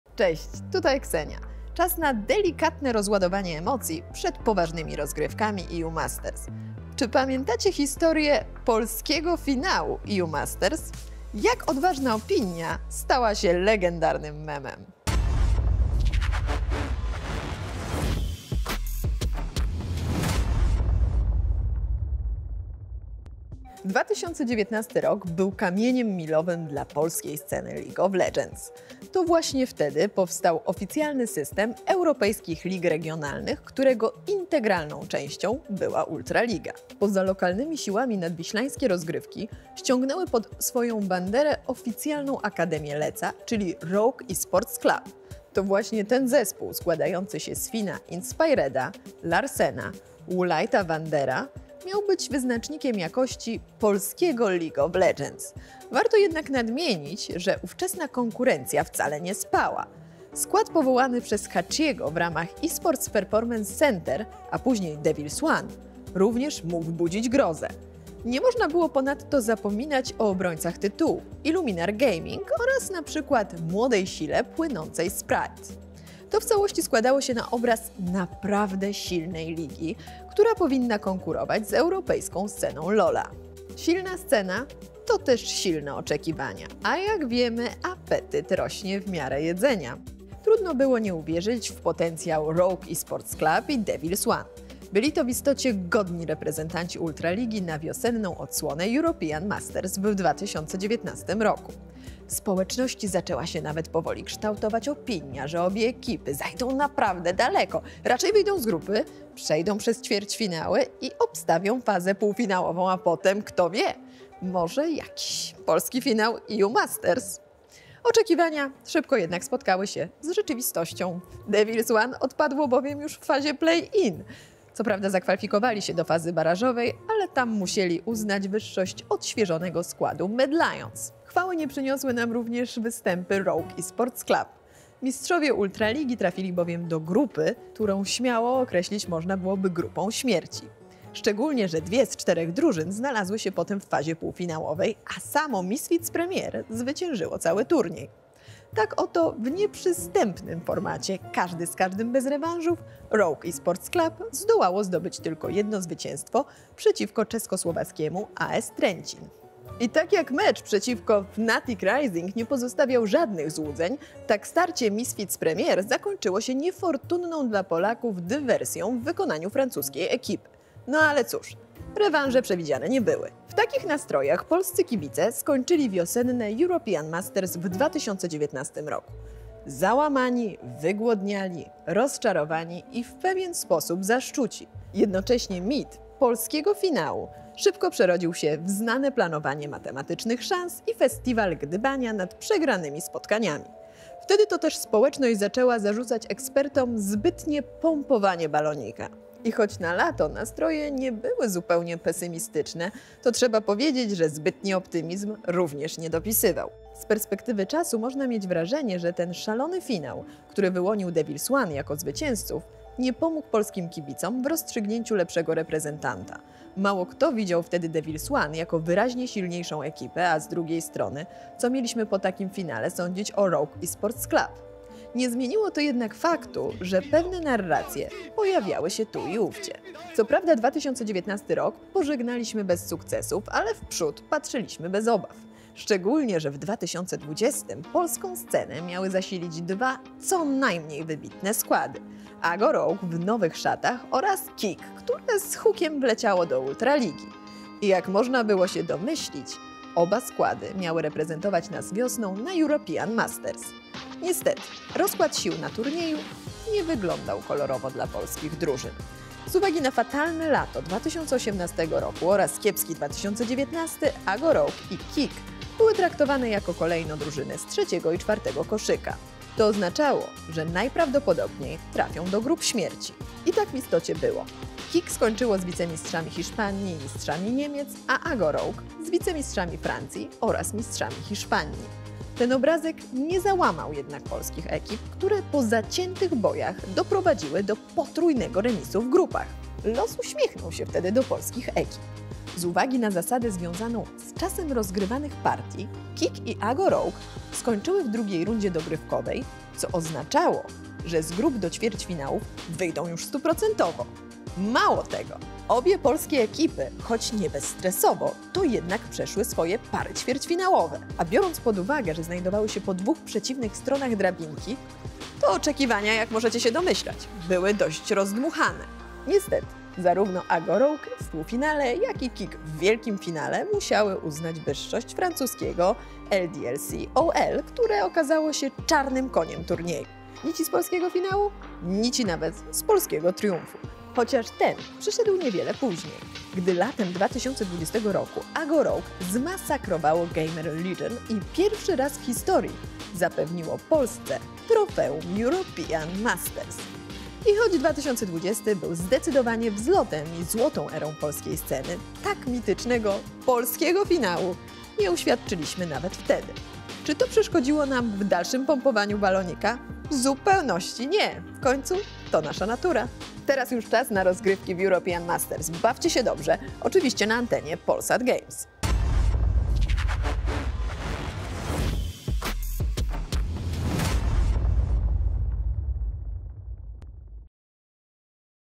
„POLSKI FINAŁ”. Jak odważna opinia stała się memem?
O nietypowym, ale jakże lubianym memie i pompowaniu balonika na EU Masters opowiada Ksenia. 🤓

TV: Cyfrowy Polsat: 16, 169 / Netia: 78 / UPC: 507, 563 / Canal+: 137.

Polsat Games. I wiesz co jest grane. 🎮

EU MASTERS 2022
FRENZY 2022